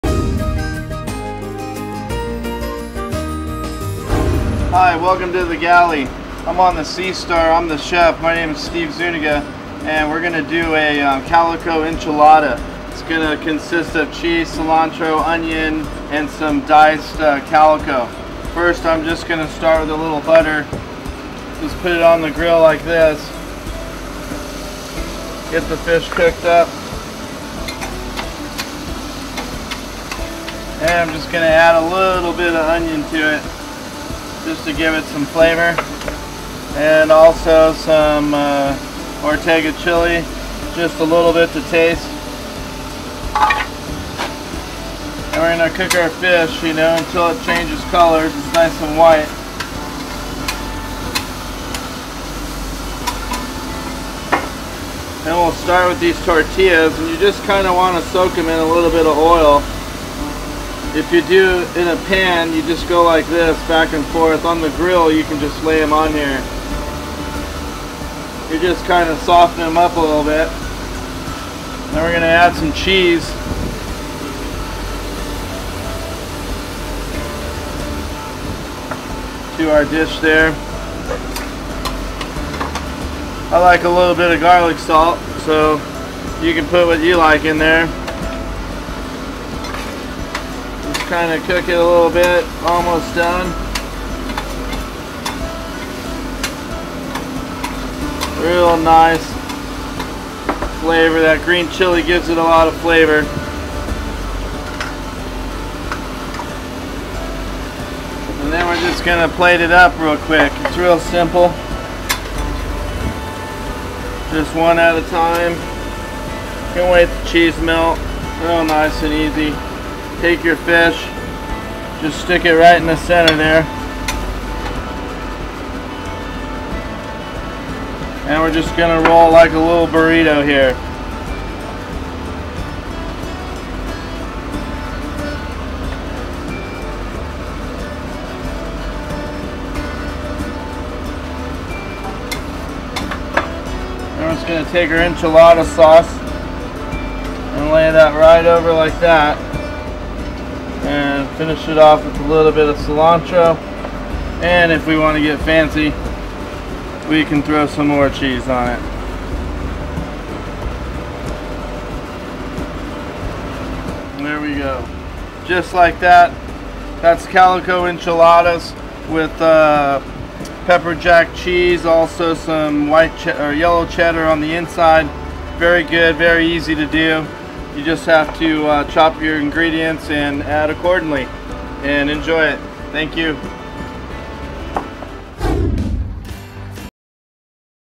Calico Bass Mexican Dish | SPORT FISHING
Steve the cook aboard the Sea Star shows us a great dish to cook up with Calico Bass.